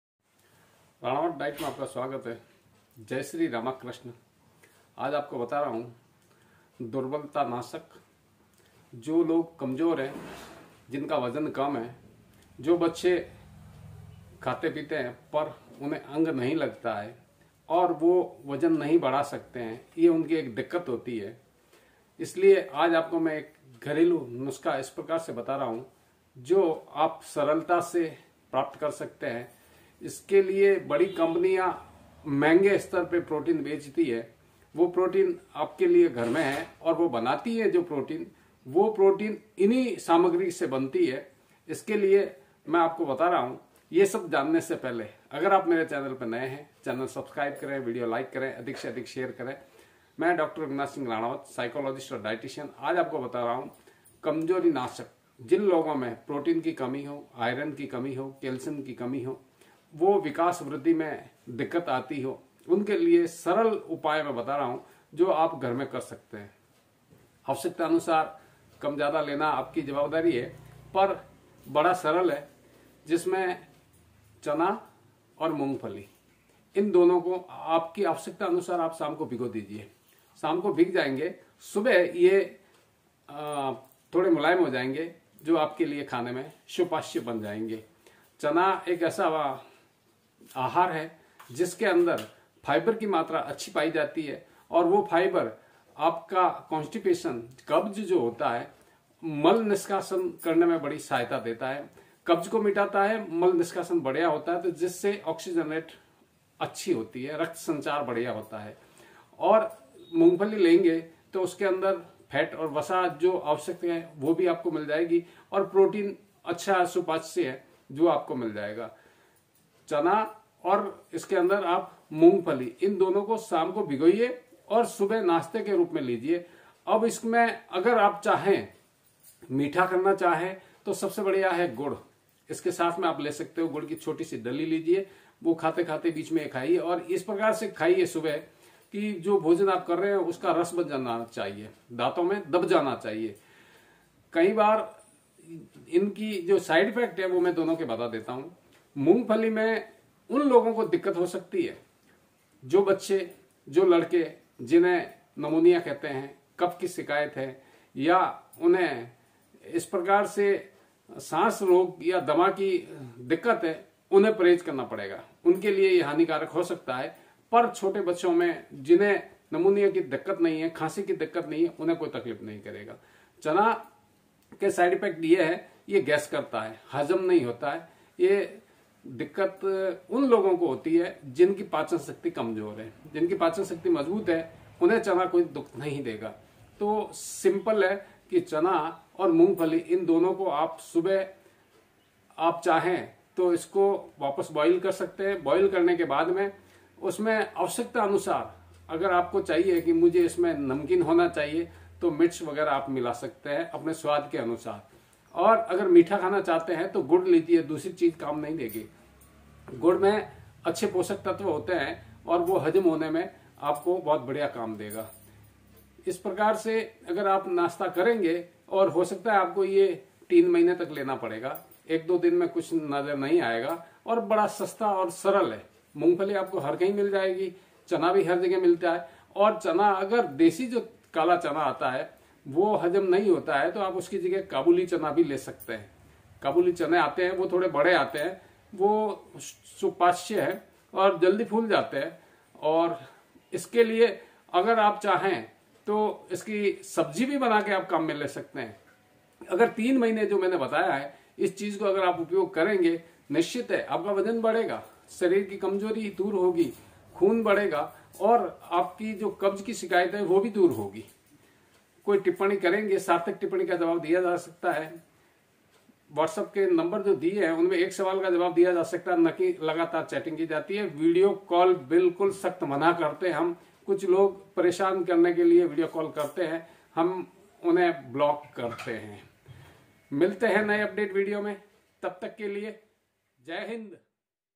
दुर्बलता का घरेलू इलाज
दुर्बलता एक जात की कमजोरी दुर्बलता के कारण चक्कर आते है, दुर्बलता के कारण भोजन नहीं करने की ईच्छा होती है, शरीर कमजोर होता है । दुर्बलता को
 दूर करने करने के घरेलू उपचार को अधिक जानकारी प्राप्त करने के लिए ये वीडियो अवश्य देखें ।